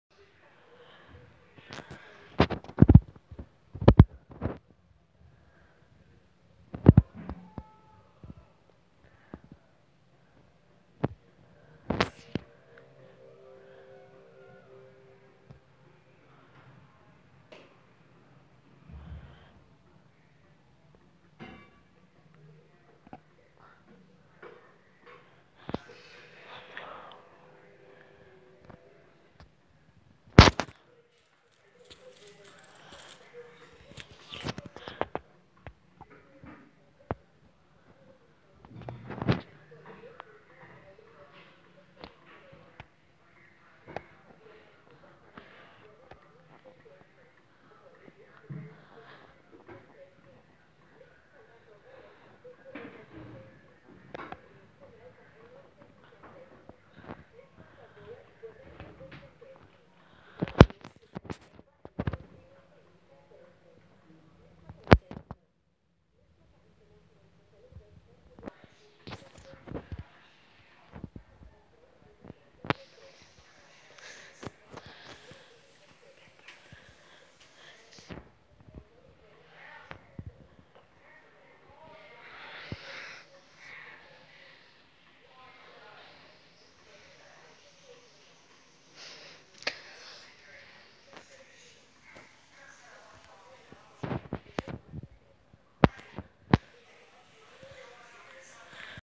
Wirja privata fid dar tieghi.
Hemm statwi ta spanja u ta malta.